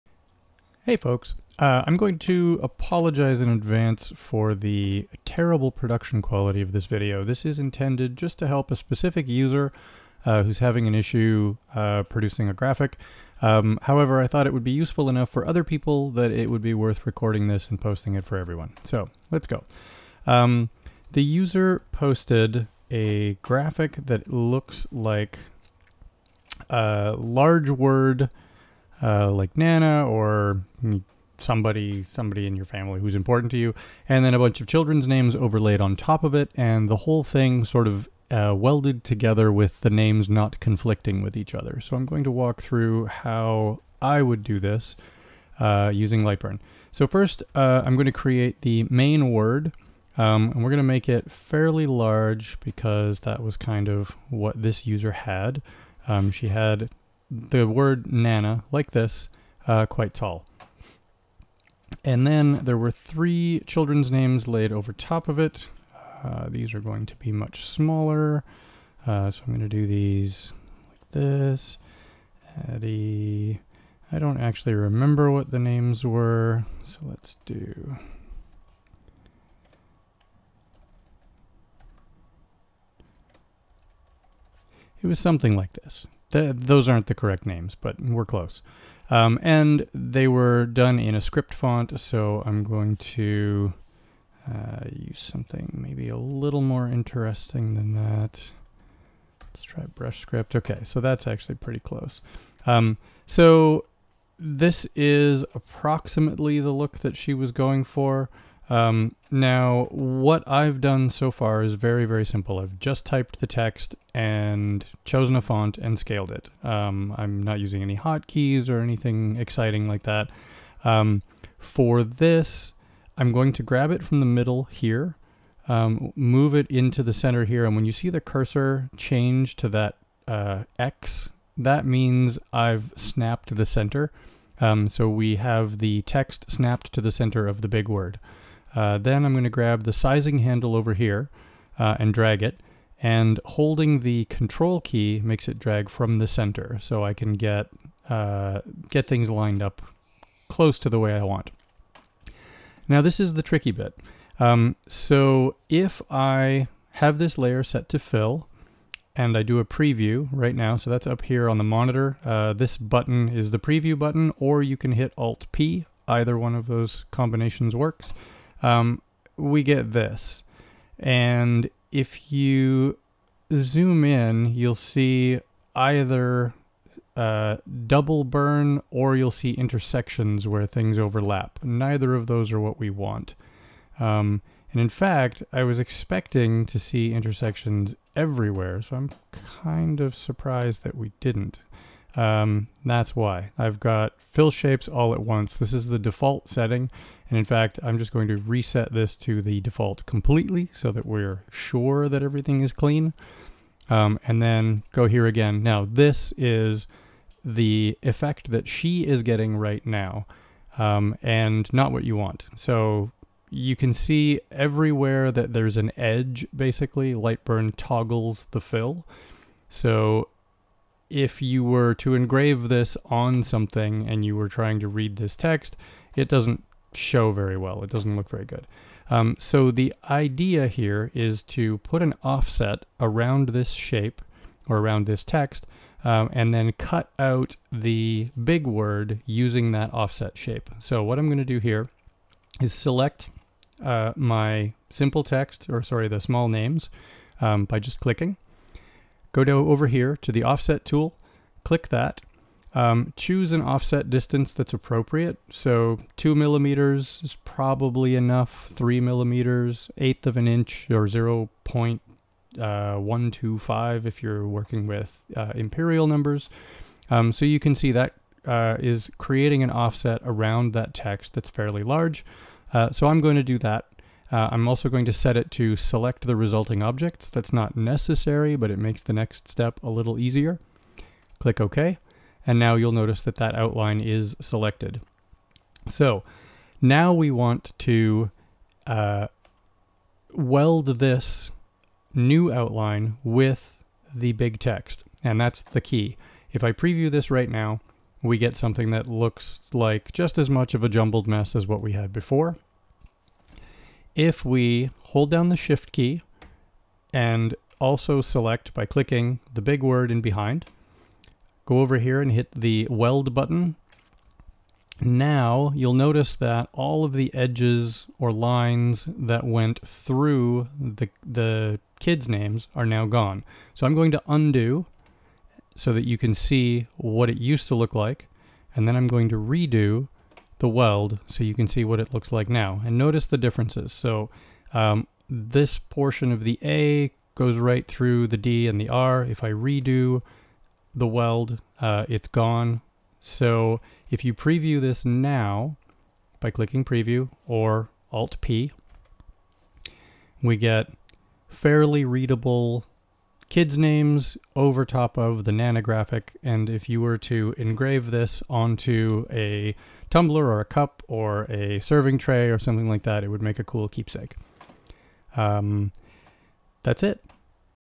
Welded Names - Quick Tips
A quick walk-through showing how to use the text, offset, and weld features in LightBurn to create a simple composited graphic.

If you need help, please join us or email support at LightBurnSoftware dot com.